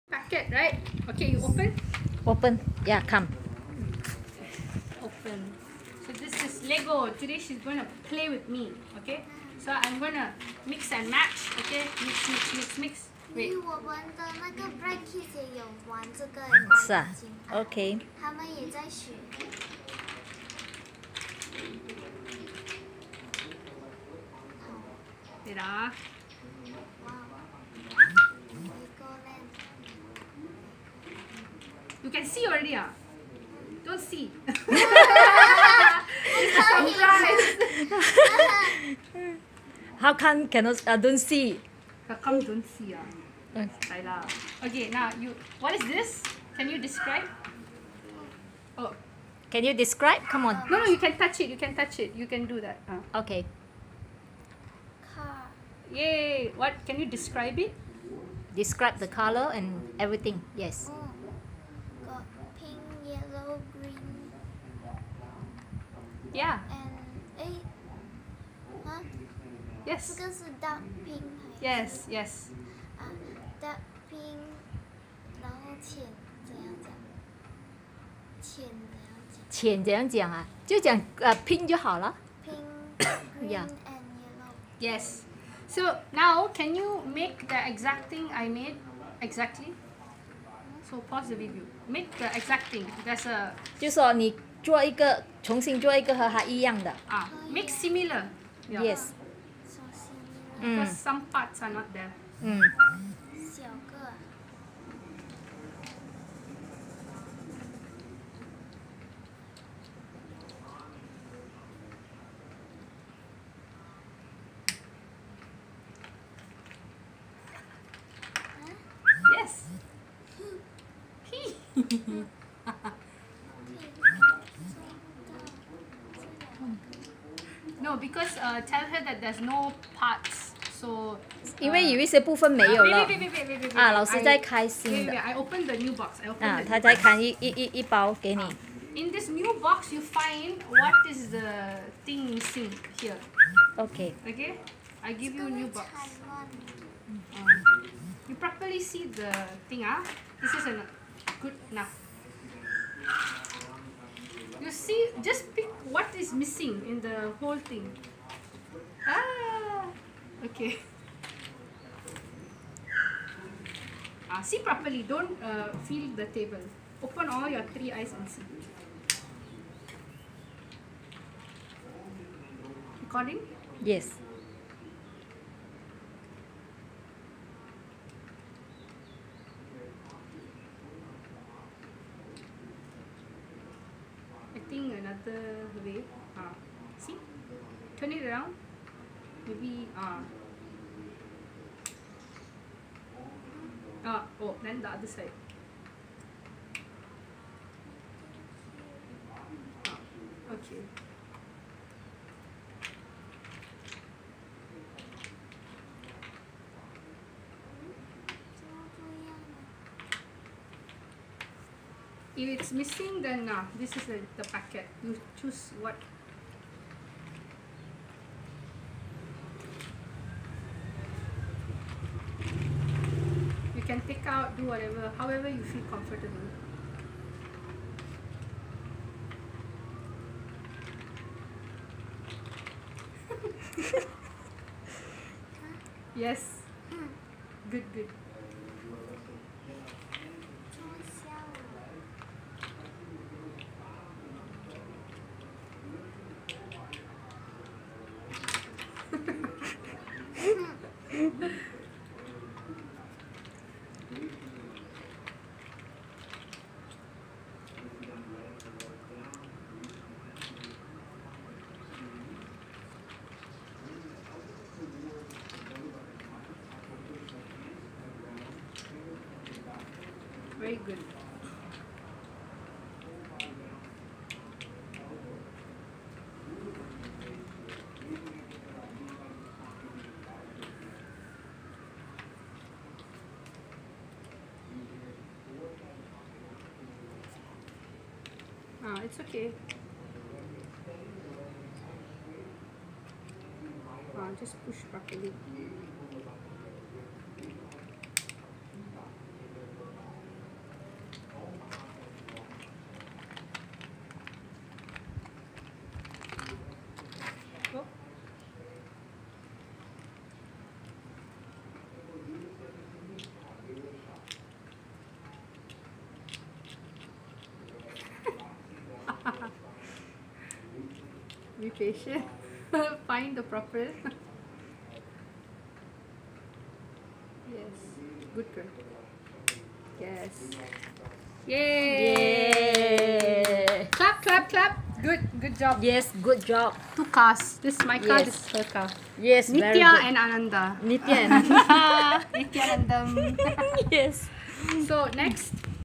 用第三眼玩 Blocks
看看经过 Bhagavvad Sri Nithyananda Paramashivam Swamiji 点化的孩子如何显化超能力……